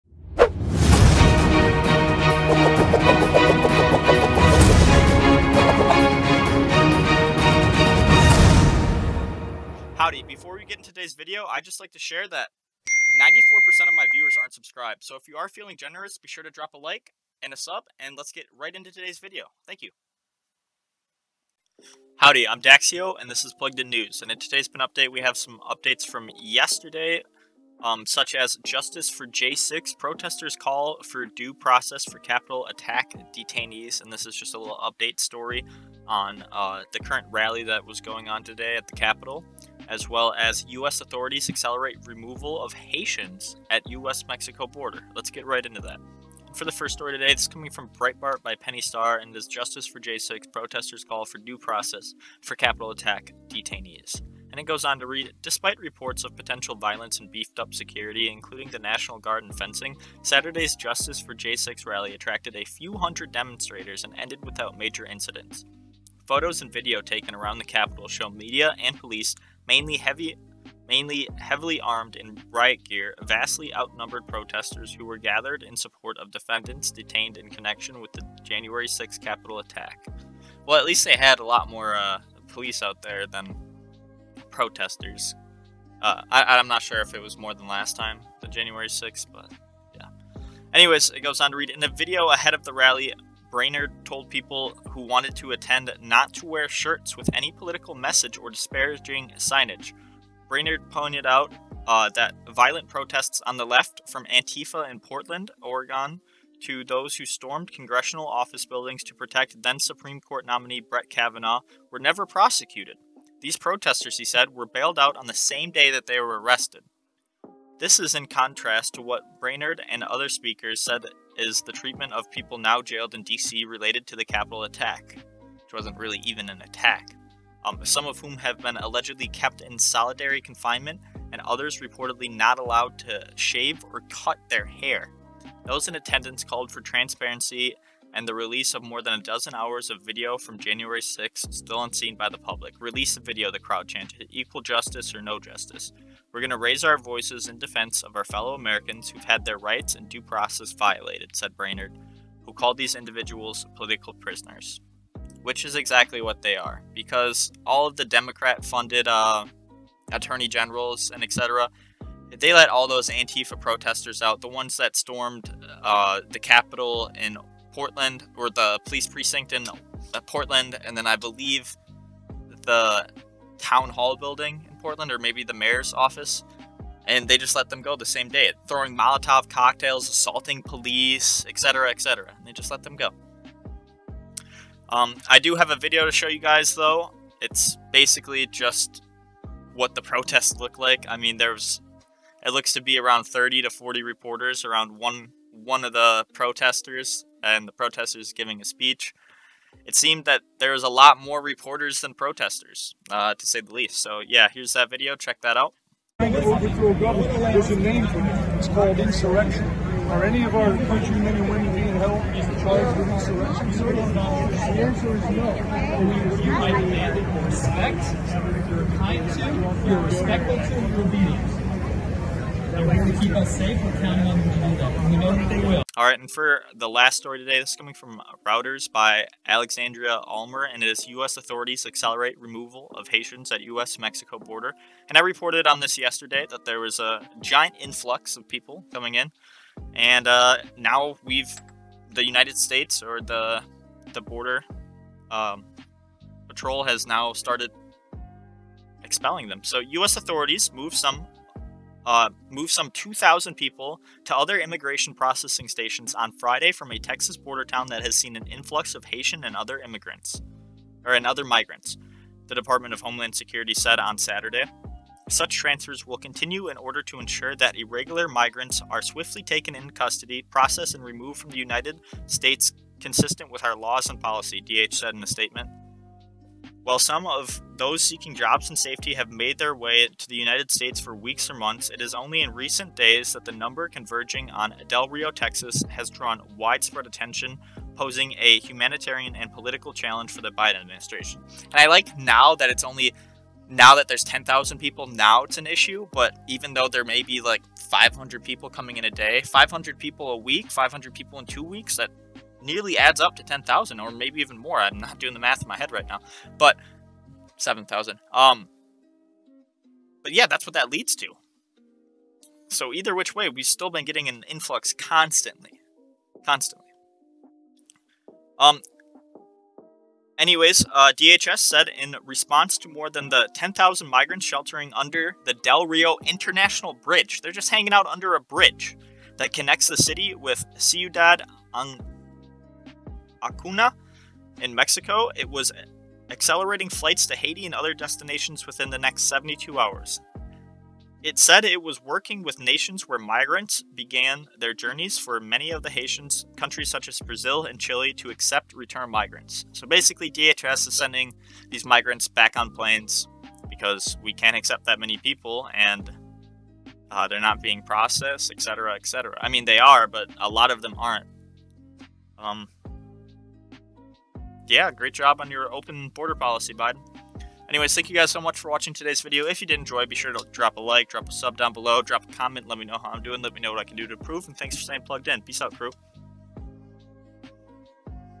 9/18 PIN Update - Protest At Capitol Smaller than Expected +VIDEO, DHS Sends Haitian Migrants Back
title has it all boys, hope you enjoy, drop a sub and a like!

‘Justice for J6’: Protesters Call for Due Process for Capitol Attack Detainees
U.S. authorities accelerate removal of Haitians at US-Mexico border

black lives matter , george floyd , antifa , joe biden , blm kabul afghanistan